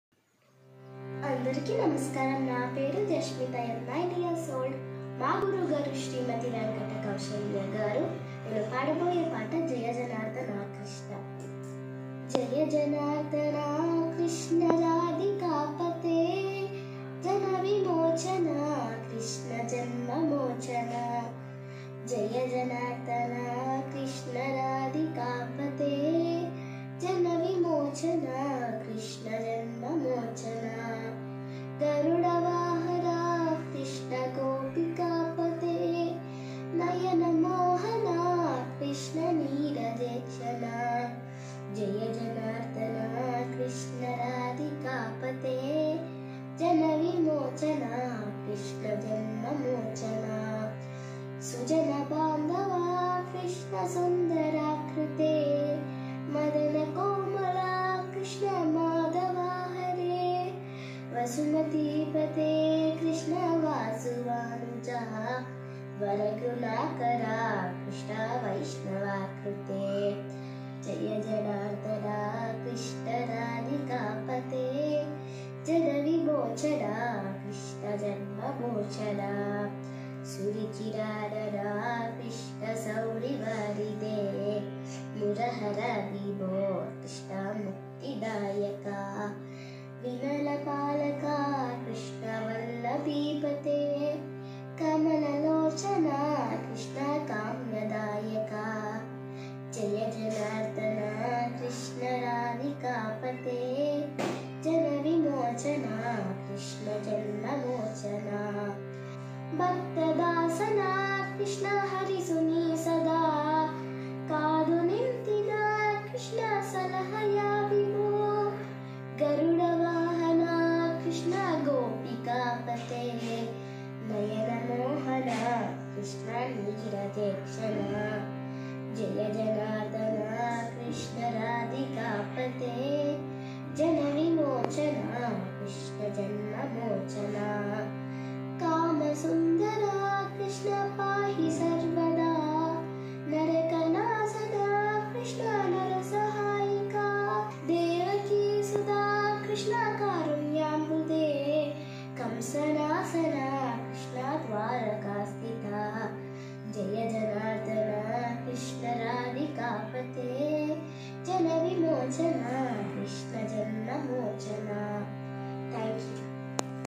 S Jasmitha | Janmashtami 2022 Online Competition | Sri Krishna Jayanthi | Sri Gaanaalayam | Carnatic
Sri Gaaanaalayam Presents
Sri Krishna Jayanthi Janmashtami 2022 Competition